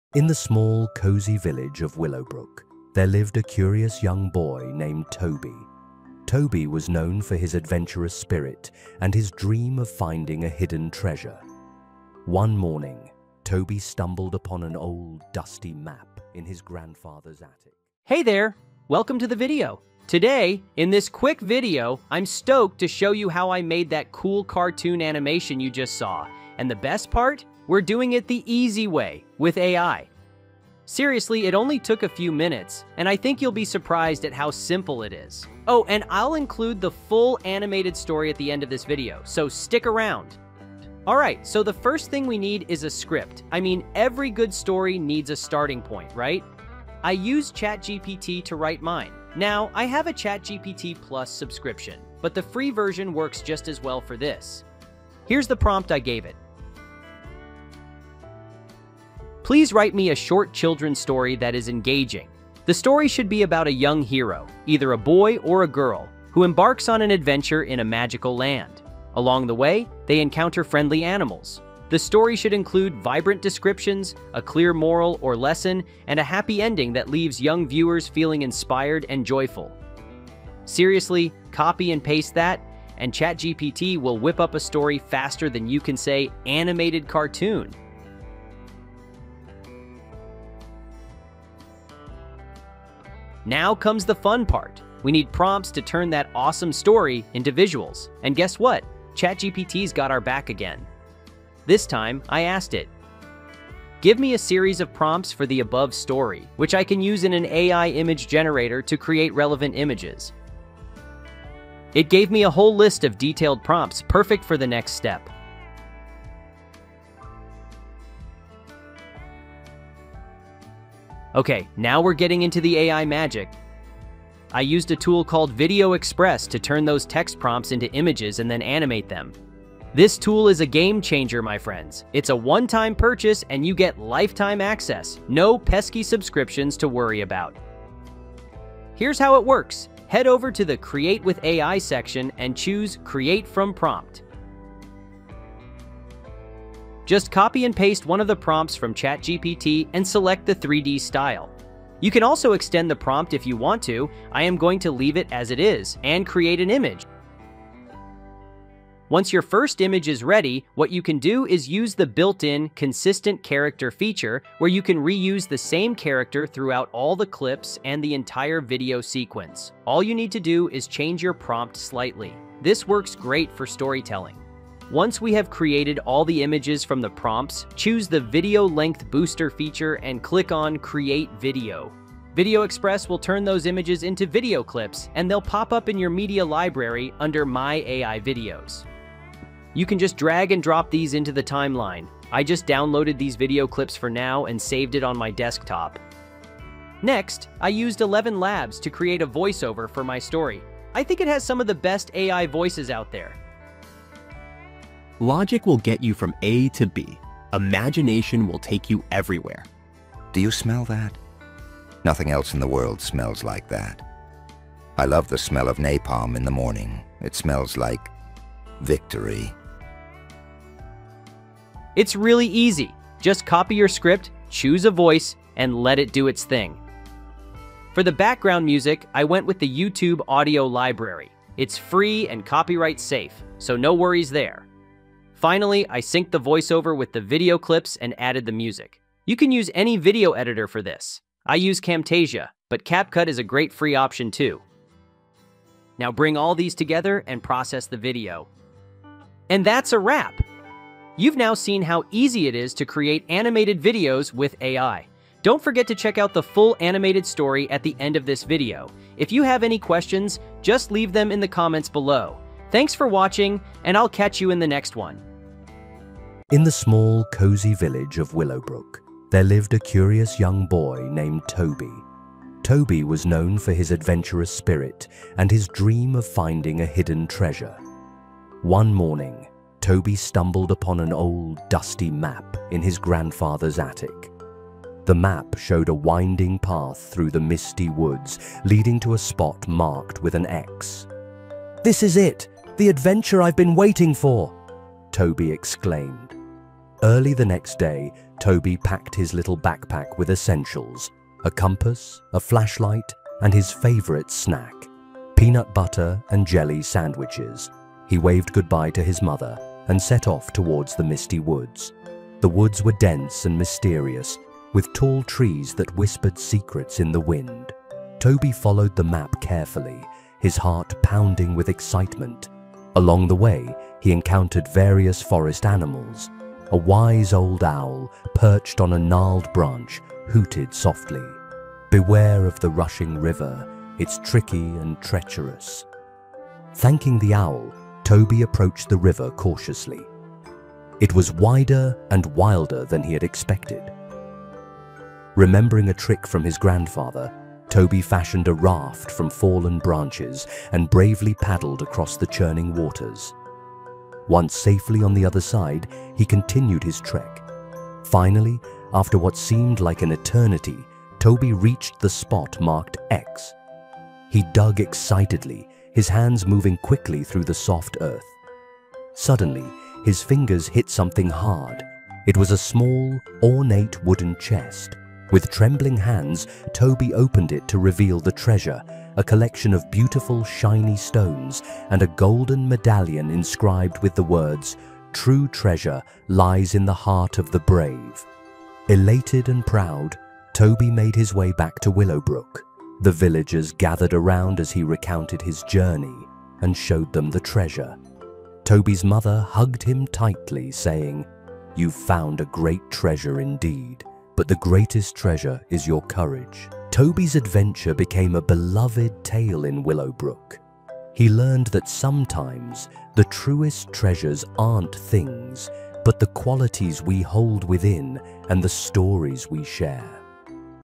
How to Make Animated Cartoon Videos with AI (Super Easy Tutorial)
Ever wondered how to make your own animated cartoon videos without needing a whole animation studio? Today, I'm showing you exactly how I made the cool animated story in this video – all using AI tools!

We'll be using a combo of ChatGPT (yes, the free version works great!), an awesome AI image generator called Video Express (it's a one-time purchase, no subscriptions!), and the amazing AI voices from Eleven Labs.

By the end of this video, you'll be able to turn a simple story idea into a full-fledged animation!

Download My Beginner's Handbook to Building an Online Business Without Being on Camera